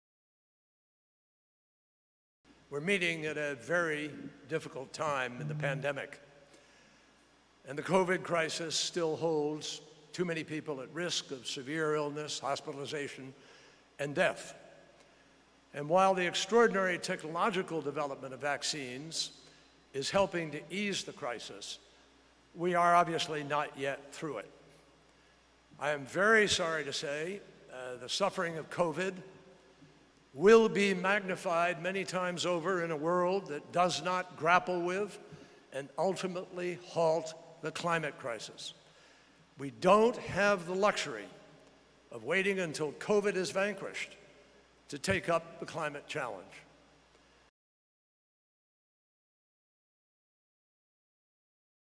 Taking Up the Climate Challenge Now
Special Presidential Envoy for Climate John Kerry: The suffering of COVID will be magnified many times over in a world that does not grapple with and ultimately halt the climate crisis. We do not have the luxury of waiting until COVID is vanquished to take up the climate challenge.